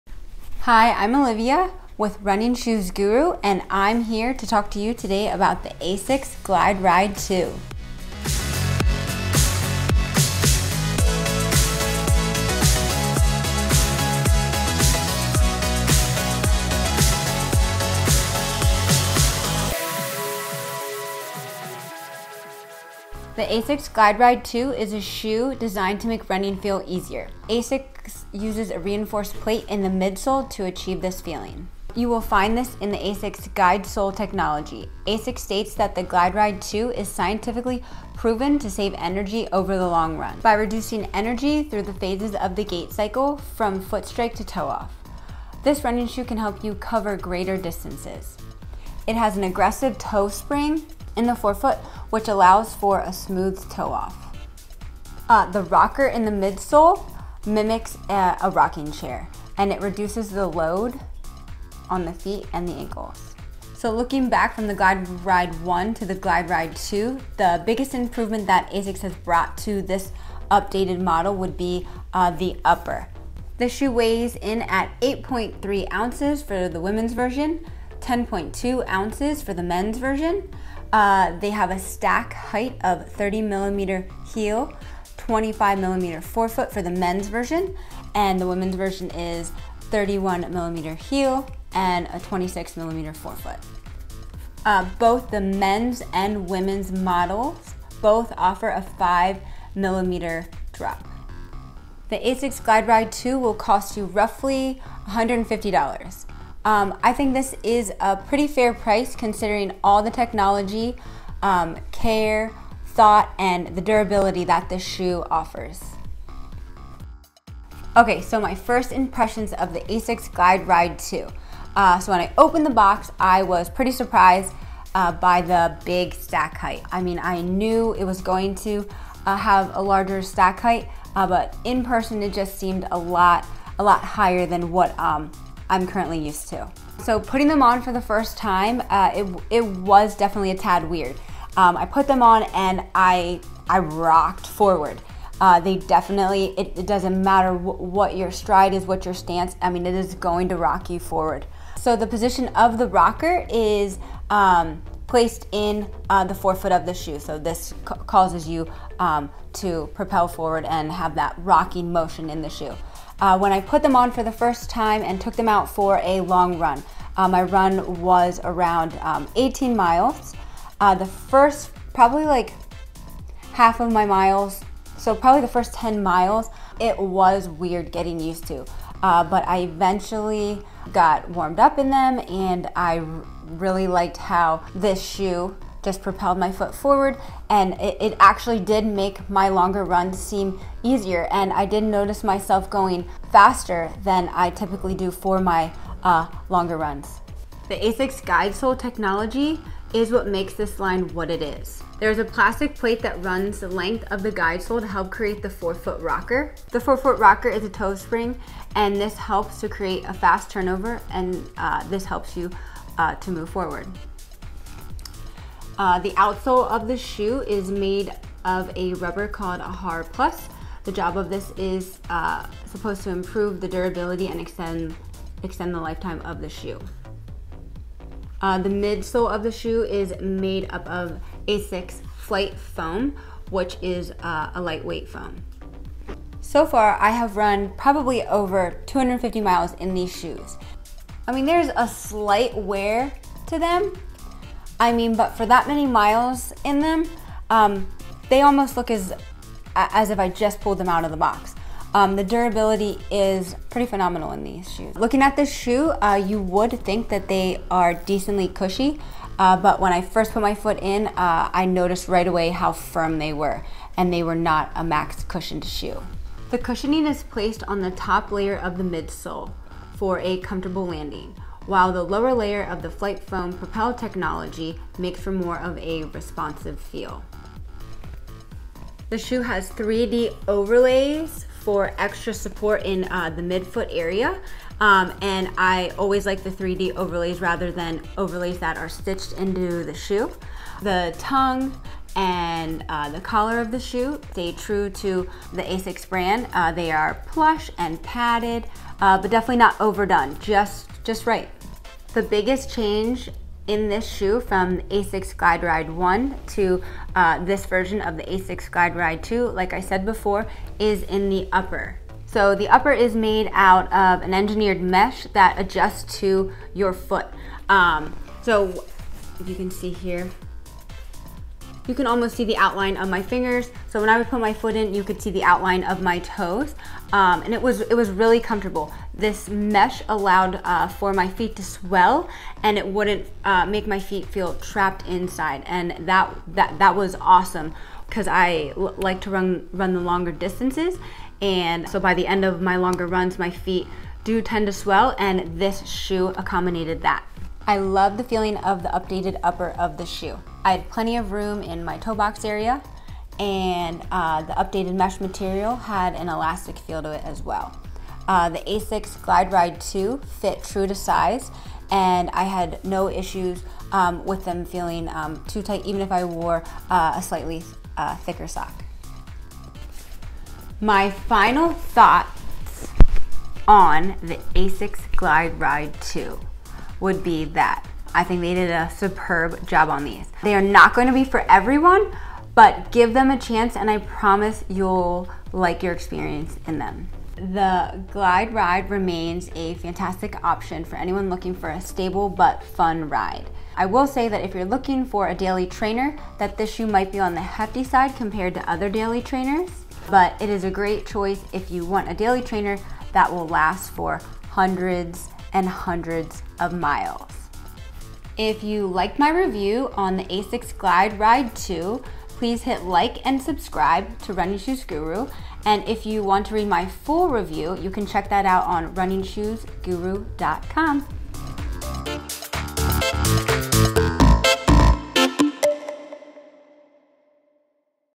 Asics GlideRide 2 - GREAT JOB ASICS !
I thought Asics did a superb job on these. They are not going to be for everyone, but give them a chance and I promise you’ll enjoy your experience in them. The new rocker technology is unique.

At first feeling, the rocker was a tad odd, but once I got used to it I grew to love it. It was a lot of fun to run in. My first “real” pair of running shoes were the Asics Gel Nimbus14.

I was hooked on Asics being my “brand.” I ended up giving up on Asics a few years ago because they were just lacking in keeping up with newer technologies.

The Asics GlideRide 2 has made me fall back in love with this brand. I highly recommend this shoe for anyone who is looking for a max-cushioned neutral shoe.

The GlideRide 2 does exactly what it says it does. It glides your whole ride. It will save you energy during your longer runs and is also a fantastic choice for recovery runs.